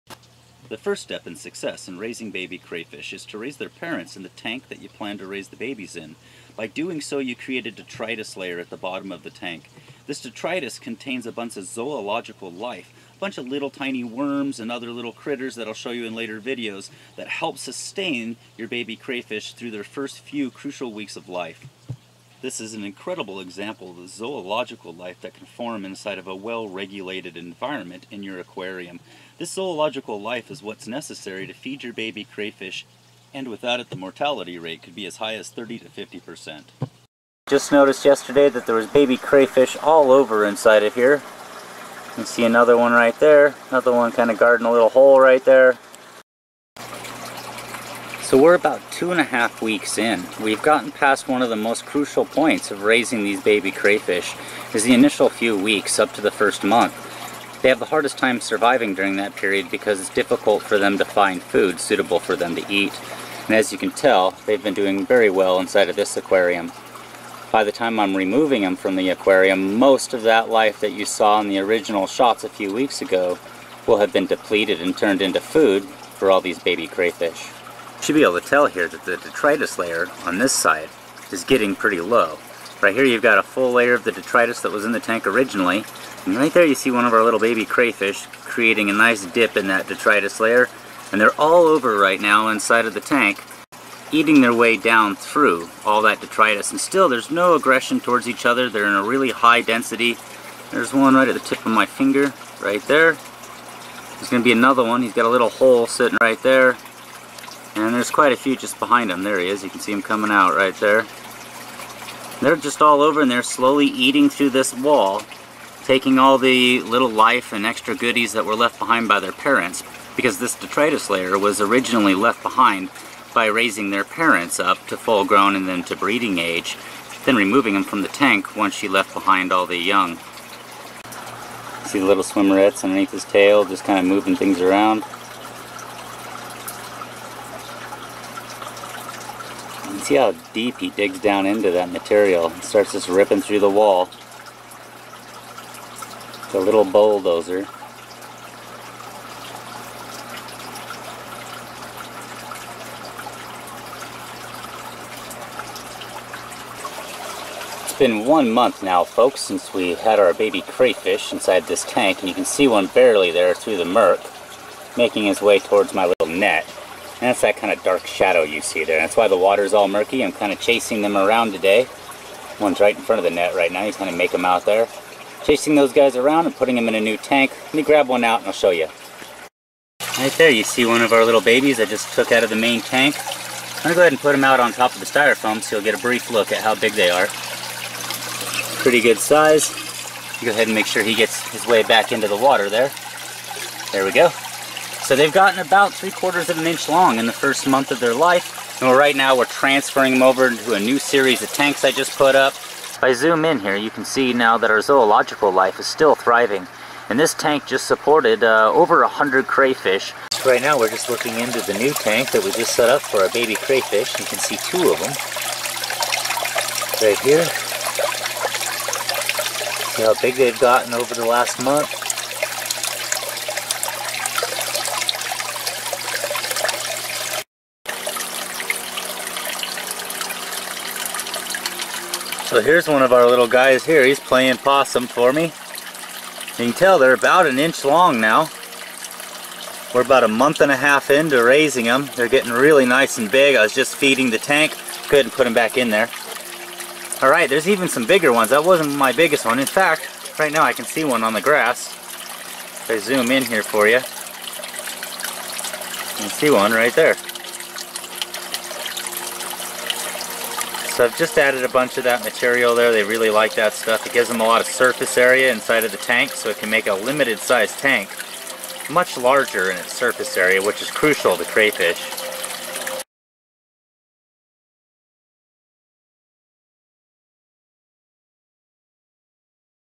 How to raise Crayfish, from 0 to 6 week's.
This is the final video in the complete how to raise Crayfish series, covering everything from preparing the tank and raising them from 0 to 6 week's. The first video in the series is " This shows the growth of the crayfish as they mature, up to 6 week's of age, and the placement and separation of them in separate tank's.